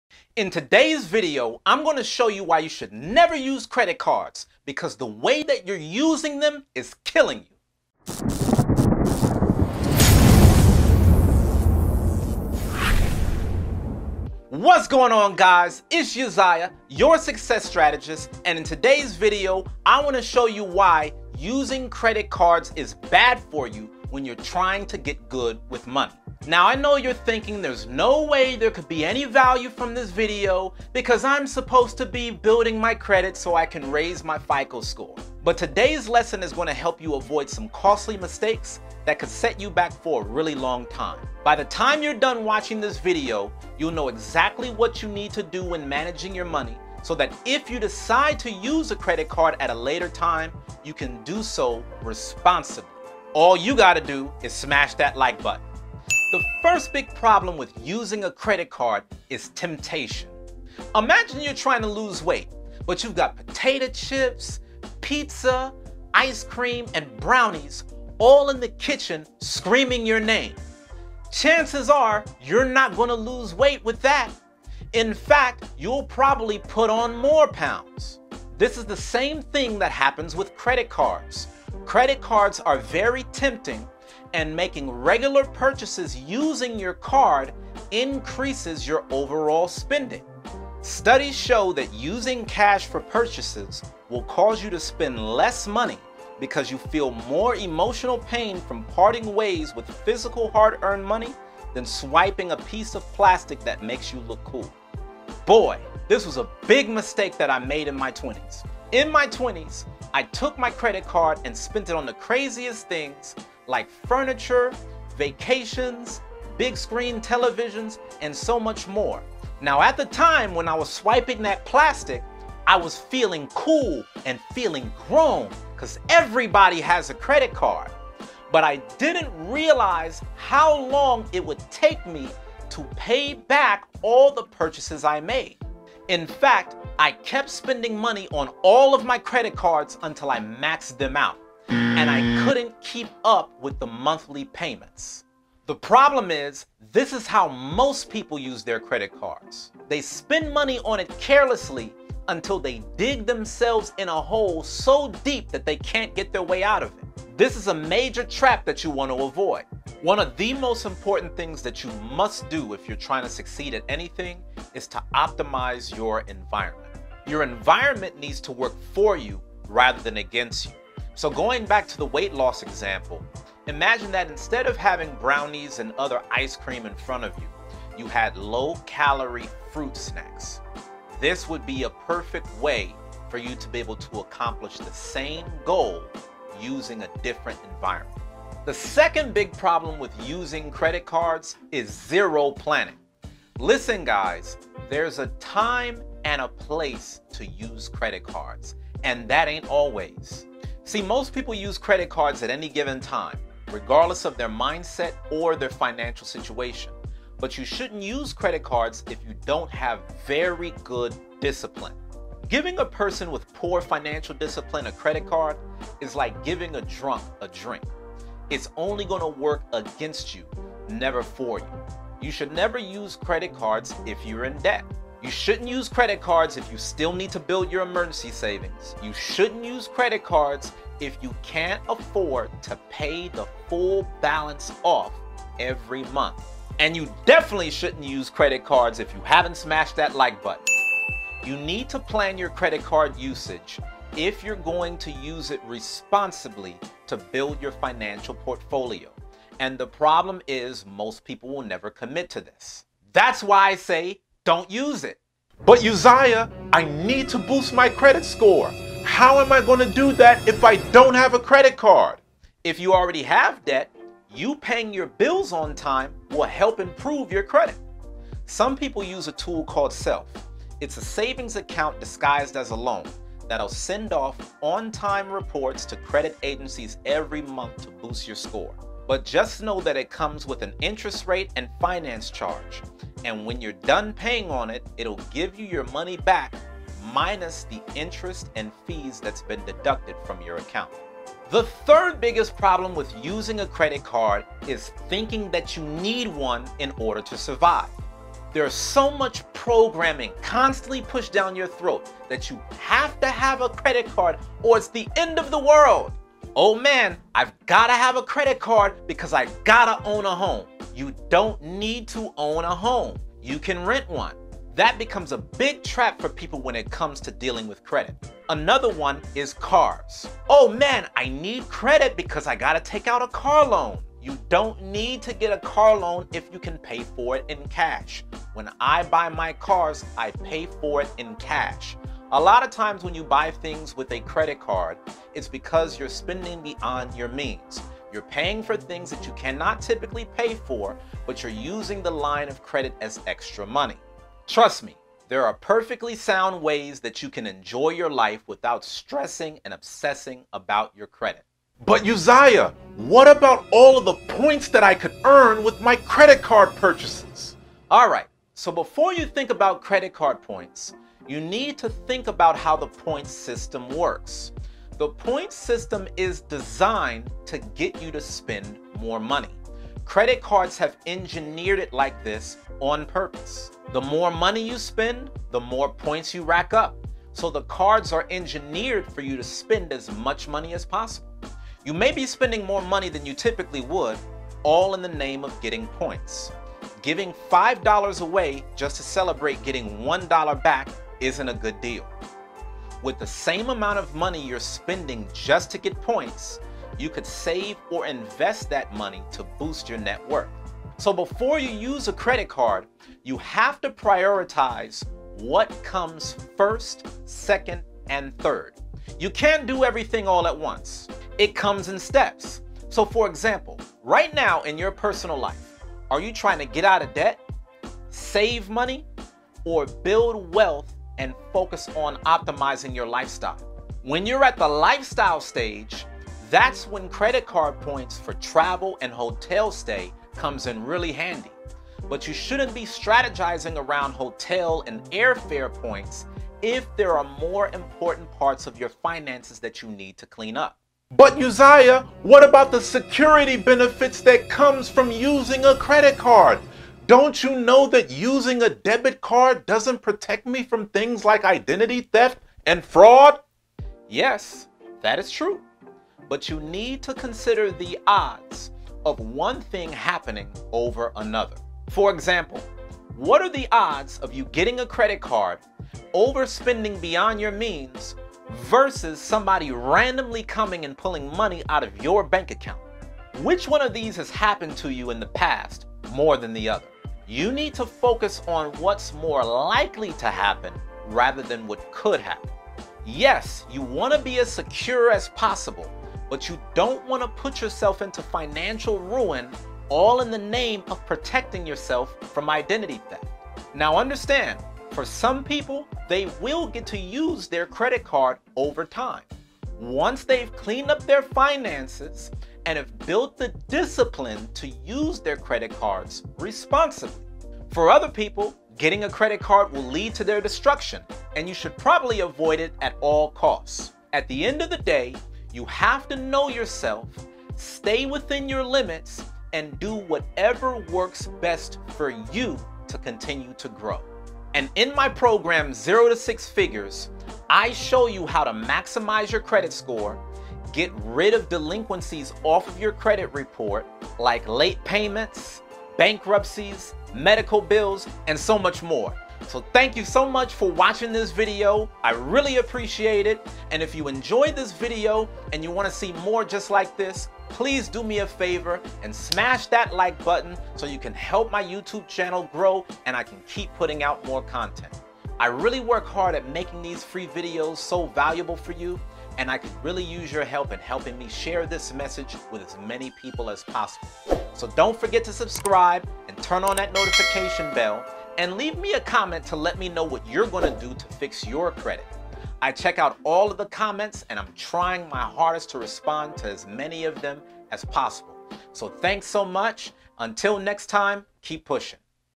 Why You Should NEVER Use Credit Cards
Add me on Instagram: BlackMensCareer

Why you should never use credit cards again (CREDIT CARDS EXPLAINED):

1. Temptation - the truth is, most people don't have the discipline to use a credit card responsibly. Unless you have really good self-control, you'll be too tempted to use a credit card on silly purchases that will only make you poor over time.

It is not wise to use a credit card to buy liabilities like clothes, gadgets, food, and other things. If you're not careful, all those little purchases will rack up to a big balance, filled with interest payments.

Does it really make sense for you to pay interest on a low-value product that you're never going to use? Do you really want to ruin your budget all because of impulse purchases? When you're trying to clean up your finances by getting out of debt, the best way for you to avoid temptation is to get rid of your credit cards altogether.

Here's one thing you need to know for credit cards for beginners: do NOT get a credit card if you're not ready to pay your balance off every month in full! Don't get a credit card just for the sake of having one. This is the quick way to ruining your FICO score, and it will take you years to fix your mistakes.

If you don't have the discipline to use a credit card responsibly, you'll rack up bad marks on your credit reports like late payments, charge-offs, and other delinquencies. Then, you'll have to resort to credit cards for bad credit and credit cards that approve anyone just to show a positive pattern of credit history.

Instead of doing searches on "credit cards 2021" - make sure you understand the dos and don'ts of using a credit card first. You may find credit cards with no credit check, but you need to ensure that you are only making purchases that you can immediately pay off so that interest and other fees don't rack up.

If you're looking into credit cards for kids, please don't destroy your child's credit card history if you don't have a strong FICO score yourself. Establish a healthy pattern of making on-time payments, and then you can think about your kids once you've made a habit of responsibility.

If you're looking into credit cards for teens, be careful. As a teenager, you can't be so emotionally driven that you buy up everything and treat your credit card as free money, because the mistakes you make here will carry over into adulthood. It's better to not have a credit card at all and use your money wisely than to have a credit card that you can't manage and set yourself back financially for years to come. Remember, things like buying a house, renting an apartment, buying a car, and qualifying for insurance will all be contingent upon your credit history.

2. Zero Planning - if you're blindly using a credit card with no strategy, you're setting yourself up for disaster. People who have credit scores of 800 or higher are very strategic about the purchases they make. If you're looking into credit cards for college students, you need to have a smart plan that you can stick to before ever taking out a line of credit. Keep in mind, if you're in college and you have a student loan, then you already have debt that will impact your credit score. Rather than taking out a credit card unnecessarily, paying off your student loans on time will create a positive surge in improving your FICO score.

Never use credit cards if you are in debt.
Never use credit cards if you don't have an emergency fund.
Never use credit cards if you can't pay the balance in full every month.

If you know in your heart that you'll never have the discipline to use a credit card responsibly, you should never use credit cards again. Searching for "best credit cards 2021" won't help you if you don't know how to use them in the first place!

3. Thinking you need one - while there are benefits of credit cards, the idea that you MUST have one is absolutely a myth. There are so many alternatives to having a credit card that will still allow you to boost your credit score. Some people think about buying a house with credit cards. Some people think about buying a car with credit cards. But the truth is that you don't NEED a credit card for either one. When I buy a car, I pay for it in cash. If you can't afford to pay for something in cash, then you can't afford it.

There's so much false programming that tells you to get a credit card, or your life is over. Learn the truth in today's video.